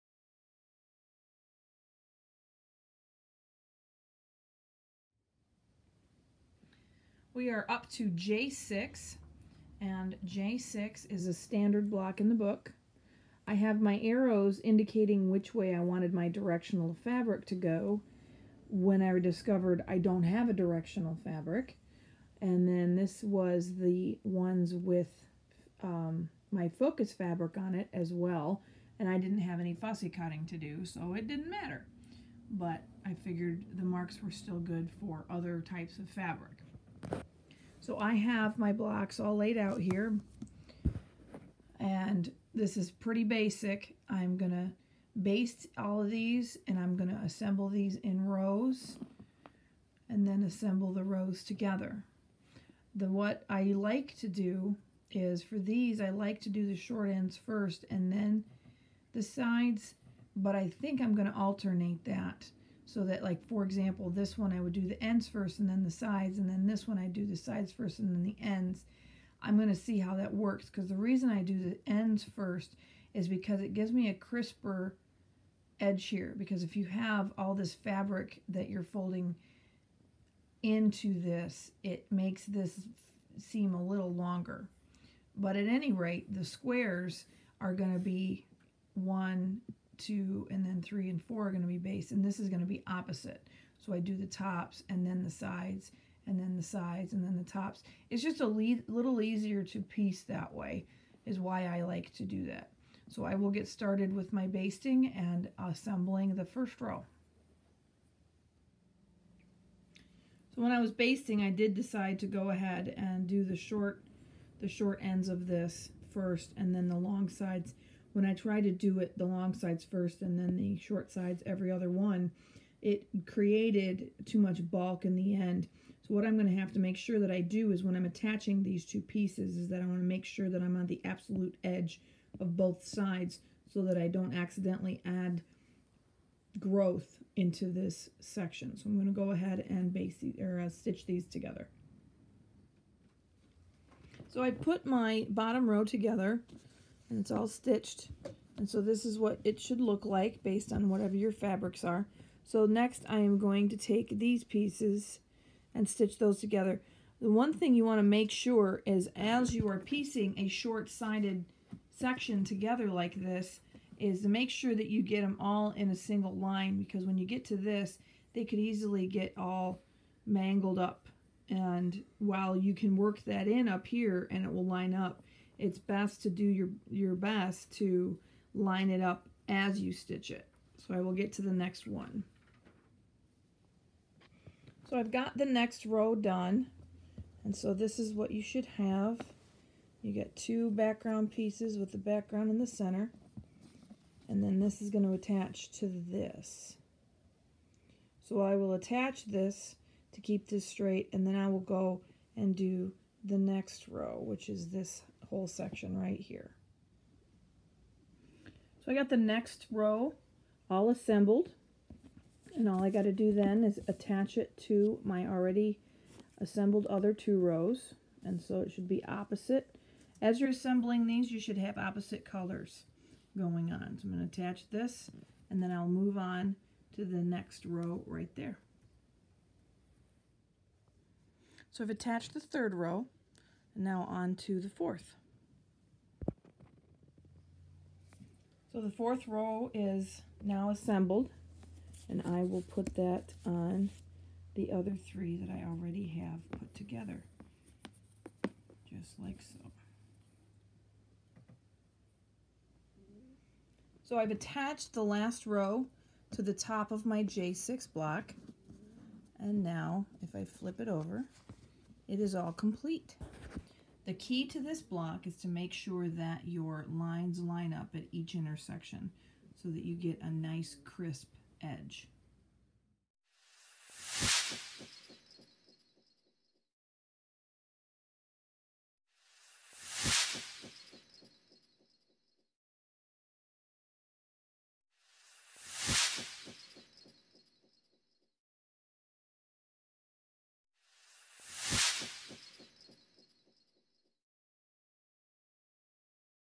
J6 Block Assembly - Dear Jane Goes EPP
Step by step instructions for assembling the J6 block of the Dear Jane quilt using the EPP kit available from Paper Pieces.